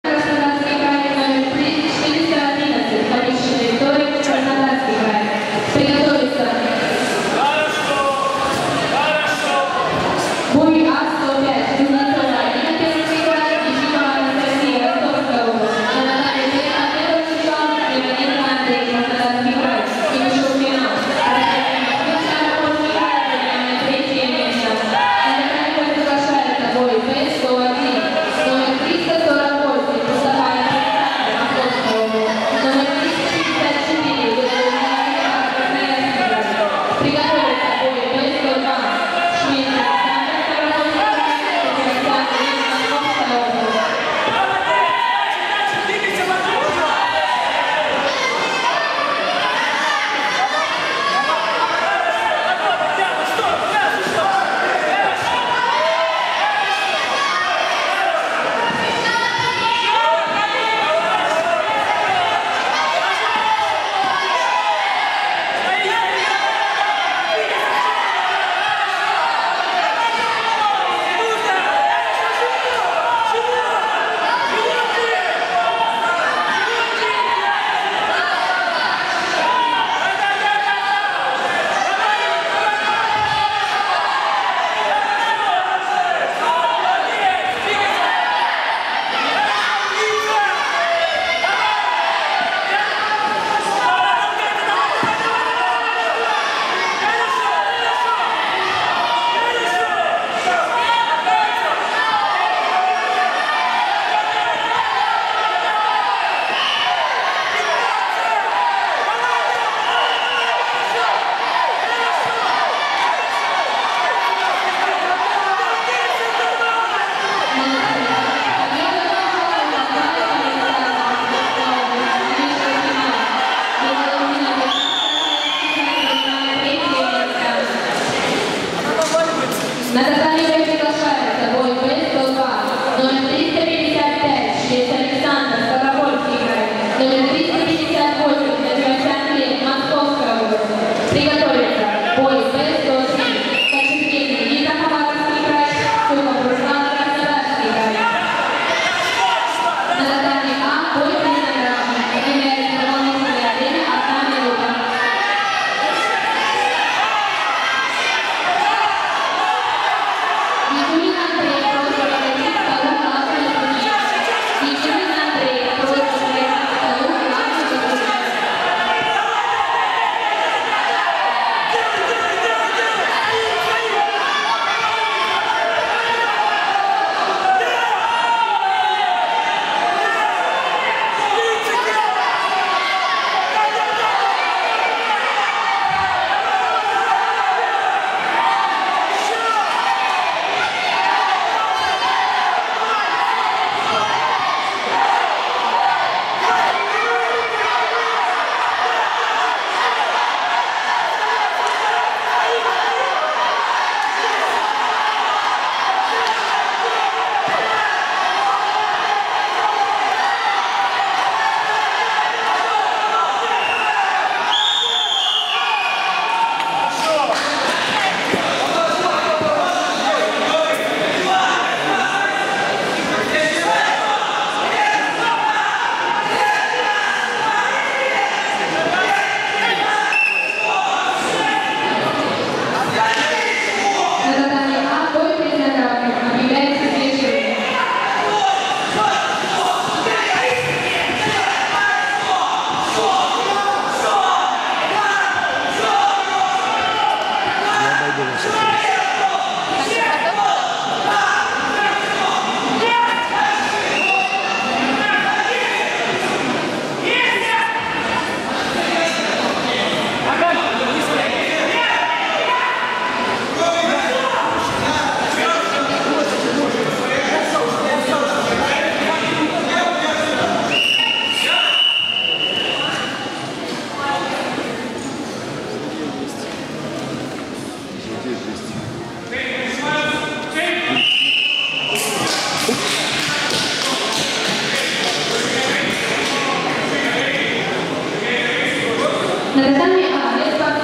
РСБИ 2017 Турпова Диана финал
В Анапе, в рамках десятых открытых Всероссийских юношеских Игр боевых искусств, посвященных Победе в Великой Отечественной Войне, с 6 по 8 сентября 2017г. прошли Всероссийские соревнования по киокусинкай среди юношей, девушек, юниоров и юниорок.